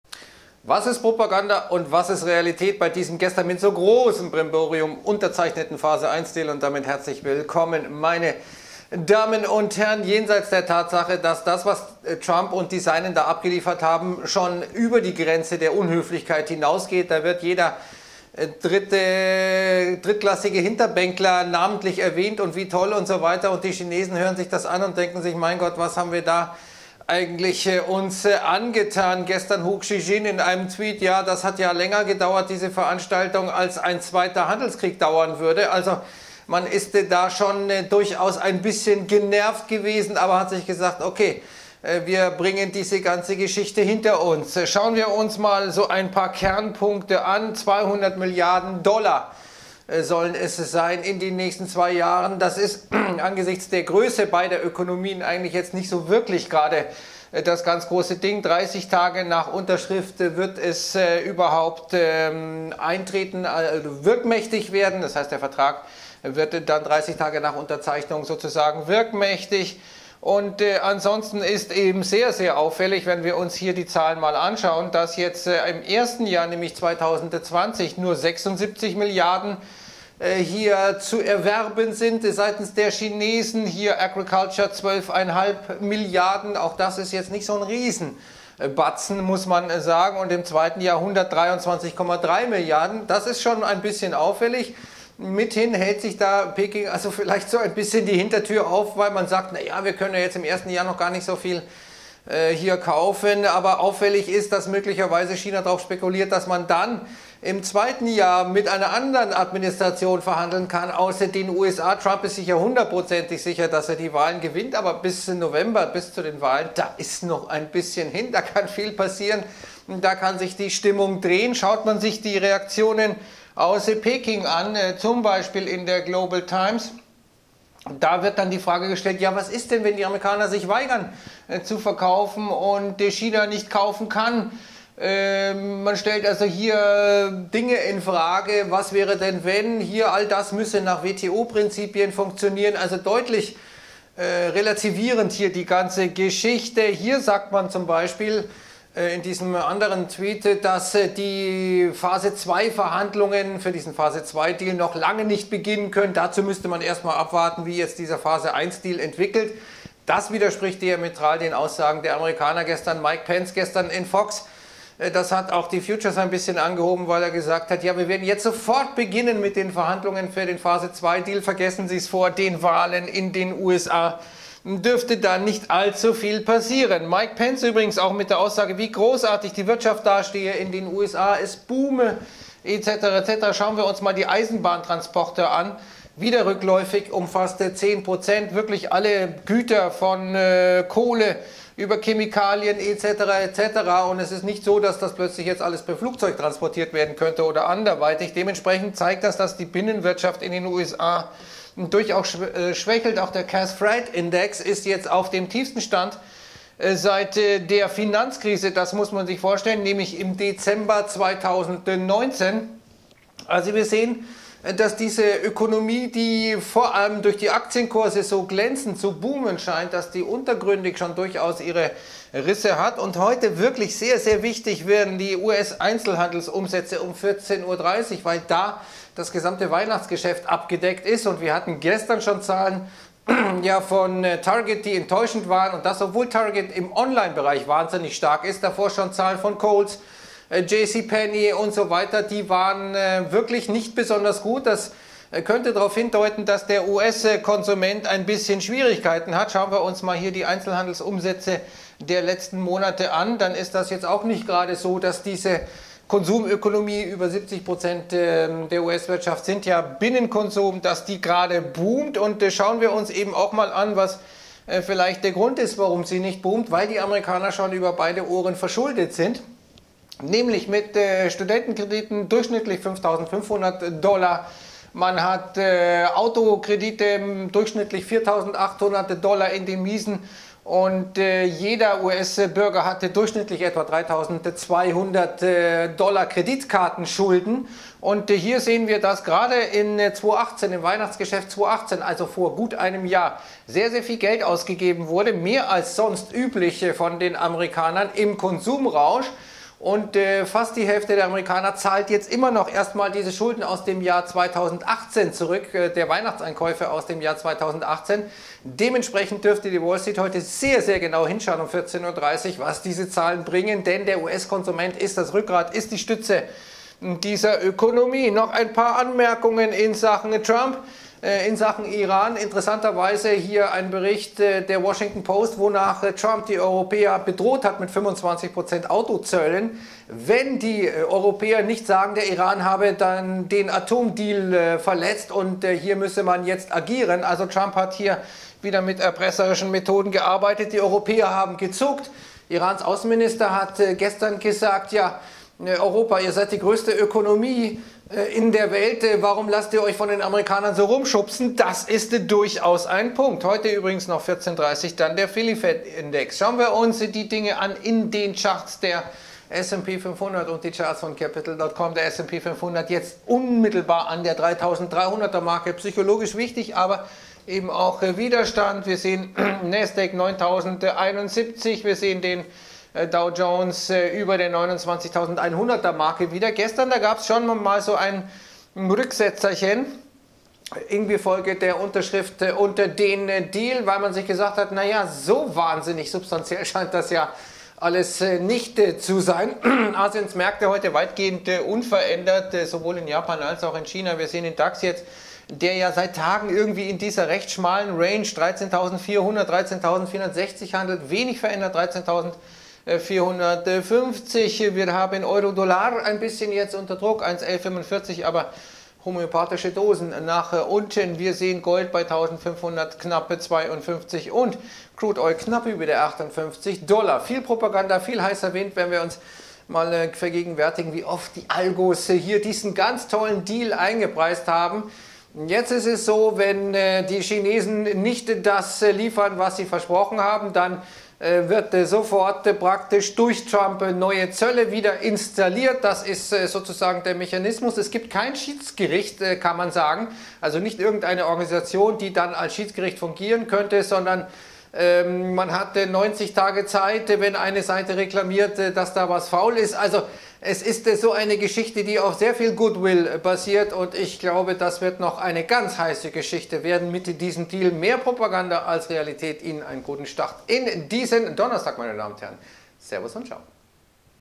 Propaganda und Realität! Videoausblick
Die Aktienmärkte haben den Phase 1 Deal zigfach eingepreist - nun aber zeigt sich, dass dieser Deal mehr Propaganda als Realität ist. Es ist ein Vertrag über Kaufabsichten, wobei auffallend ist, dass das Volumen erst im Jahr 2021 wirklich signifikant steigt. Die Aussagen aus Peking klingen reservierter, man hält sich offenkundig einige Hintertürchen offen, von einem (sofortigen) Start der Phase 2-Verhandlungen (wie gestern von US-Vizepräsident Pence behauptet) ist keine Rede. Heute wichtig für die Aktienmärkte die US-Einzelhandelsumsätze, weil die Zahlen zum Weihnachtsgeschäft zentral sind für eine Konsum-Wirtschaft. Die Zahlen der US-Einzelhändler waren jedenfalls bisher sehr mau. Wagt sich der Dax nach der Stagnation der letzten Tage heute aus der Deckung?